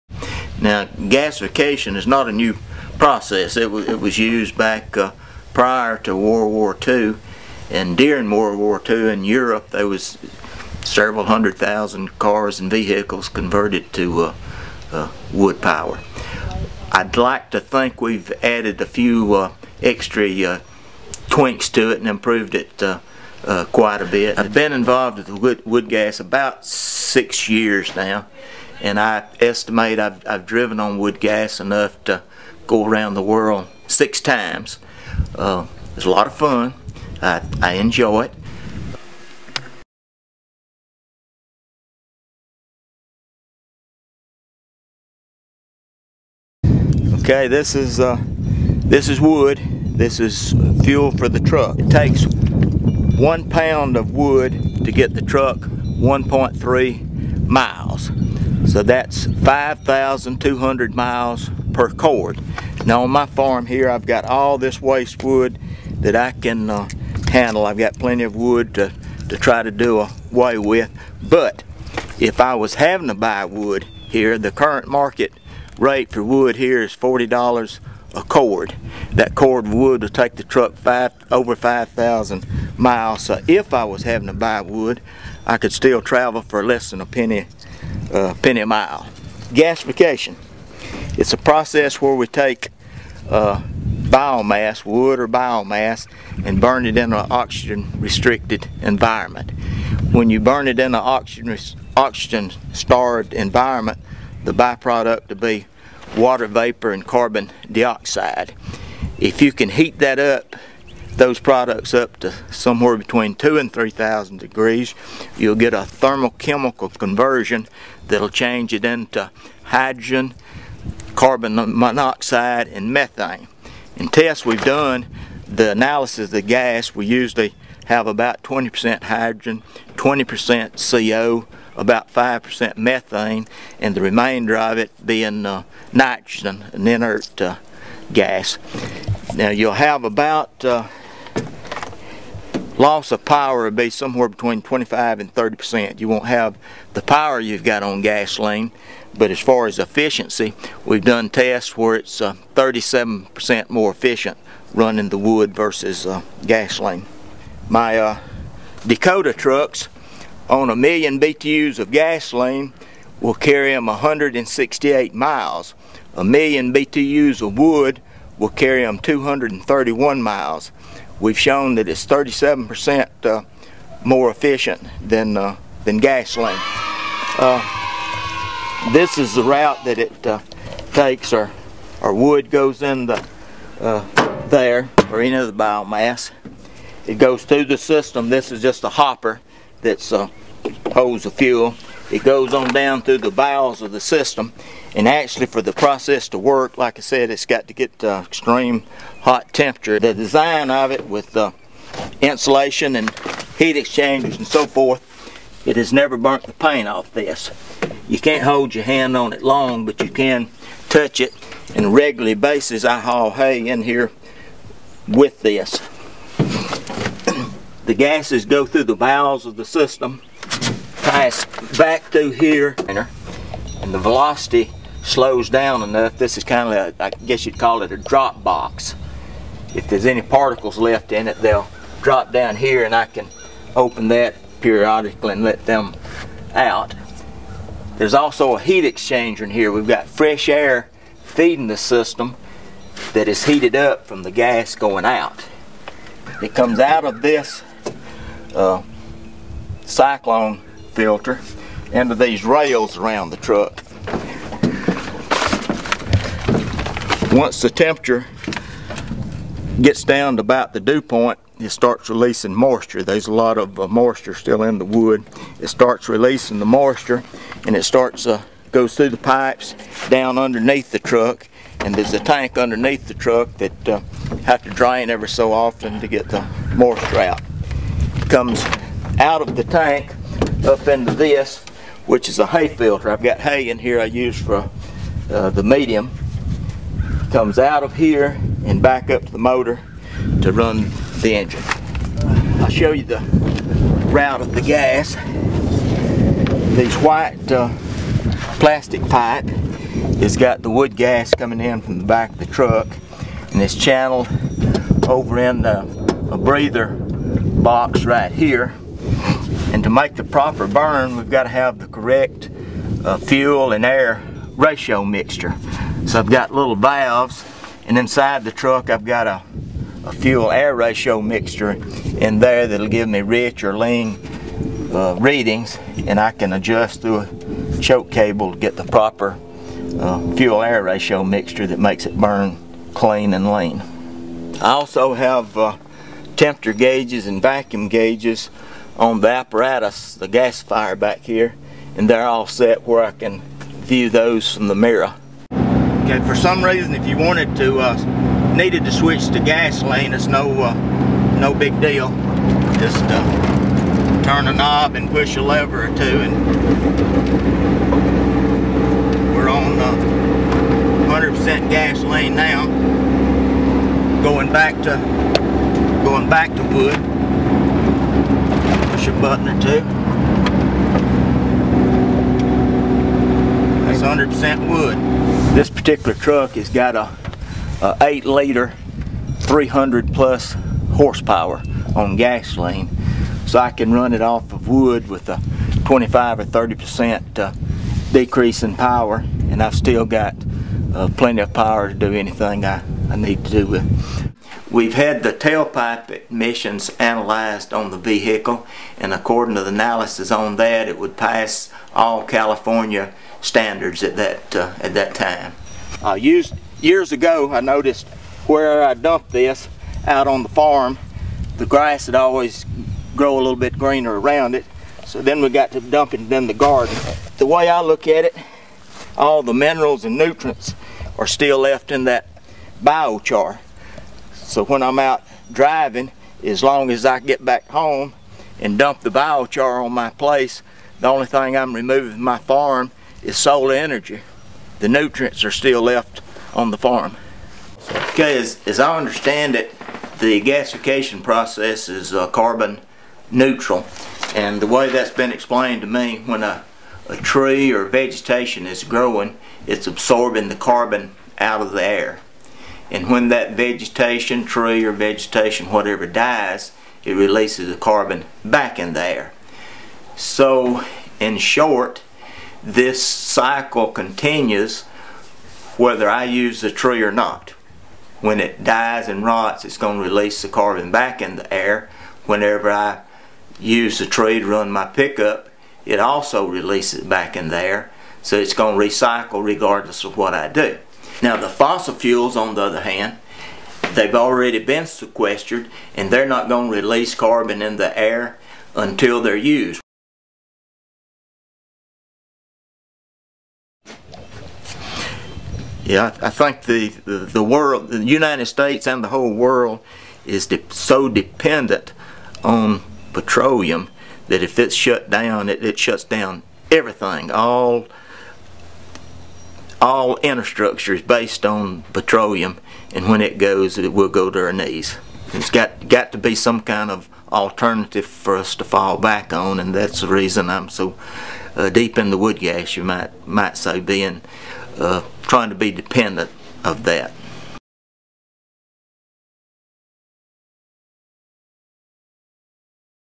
Wood Gas Part 2: Extras and In-Depth Info
Wayne Keith takes us through the ins and outs of his wood-gas powered trucks. He explains details and shares comparative efficiency stats.